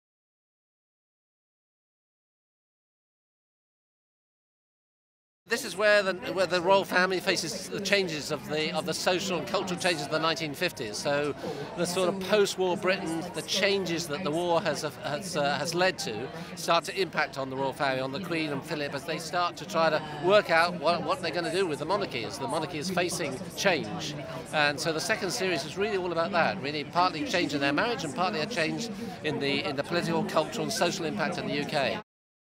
Netflix | Światowa premiera 2. sezonu serialu "The Crown" - Andy Harries | Nflix.pl
Światowa premiera 2. sezonu serialu "The Crown" - wywiad z Andy Harries Executive Producer Soundbites